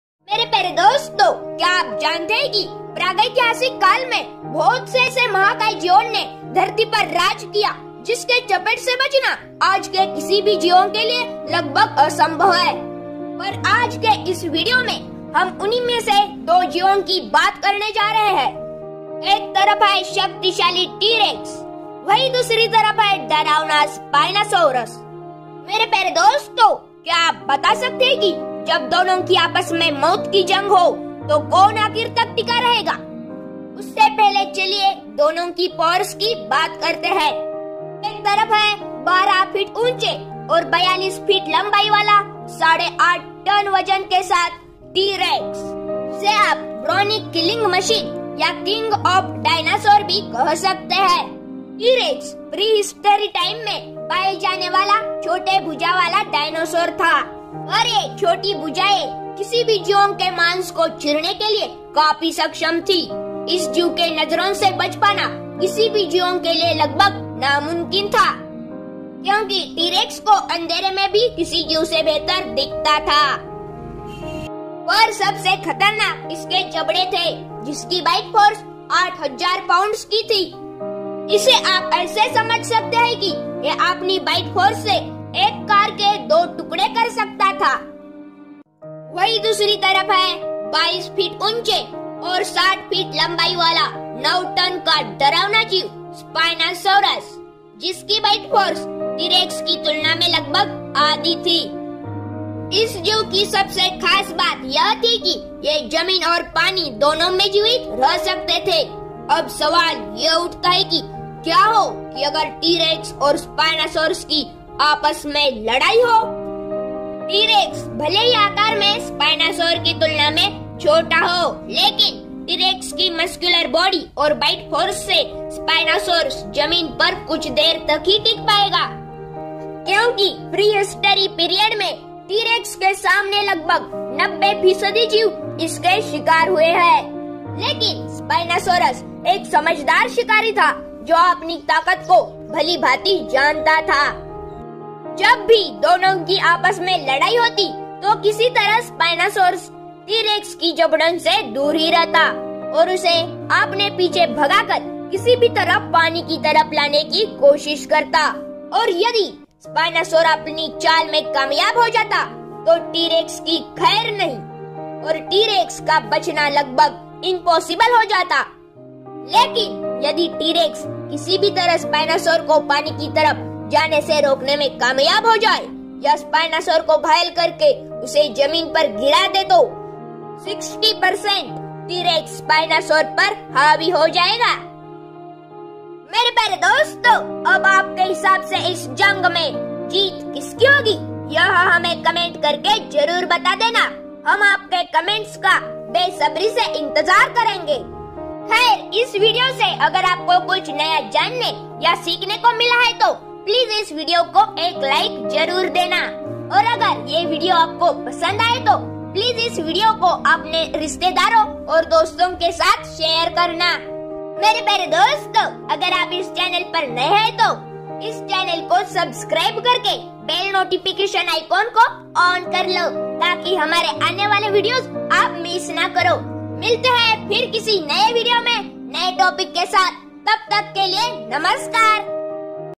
T-rex vs Spinosaurus, मौत की जंग,Wild Life animals
.                                          Hello !
         We makes very informative videos about wild      animals.
           It took nearby 10 days to complete this video
                             कौन जीतेगा इन दोनों में से ?
                                   Who will win ?
                   A T REX - OR - A SPINOSAURUS ?
                  Please, comment us we are waiting !
        If you like our videos please support us to grow.